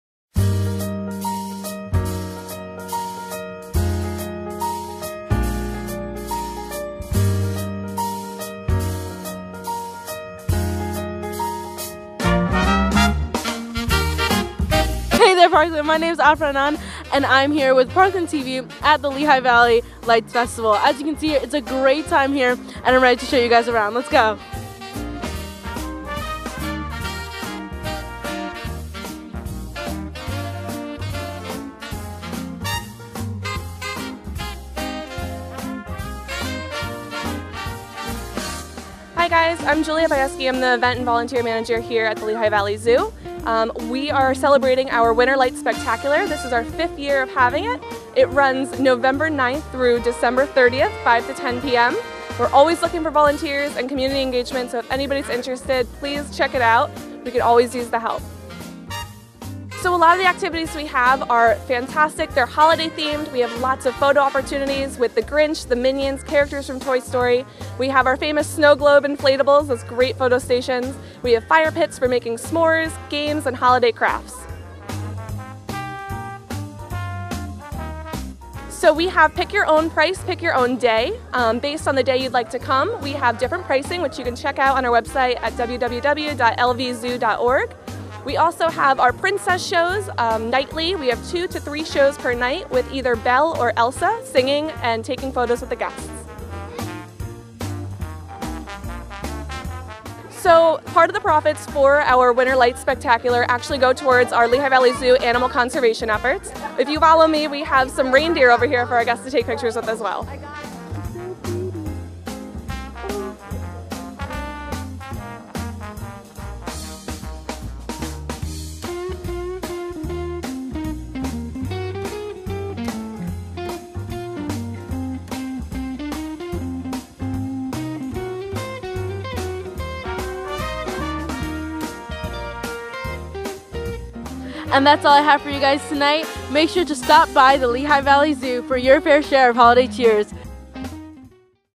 Lights At The Zoo 2018 - Lehigh Valley Zoo
Afra Anan gives you an inside scoop the spectacular 5th annual light showcase at the Lehigh Valley Zoo